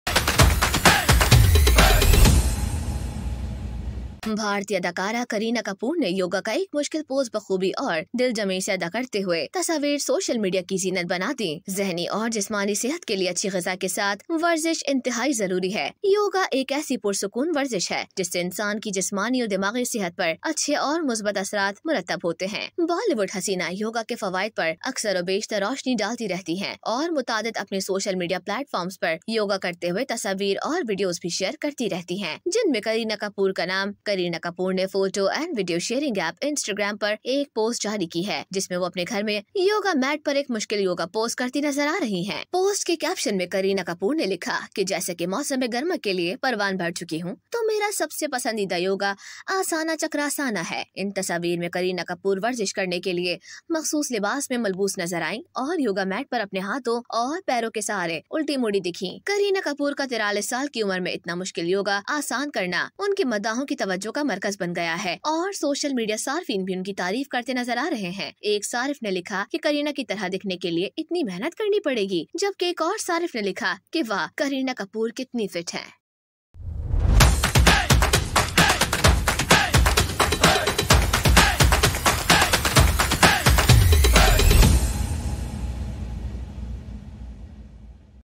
Kareena Kapoor's Viral Yoga Photos – See Her Impressive Poses | Hungama Express
Kareena Kapoor's Viral Yoga Photos – See Her Impressive Poses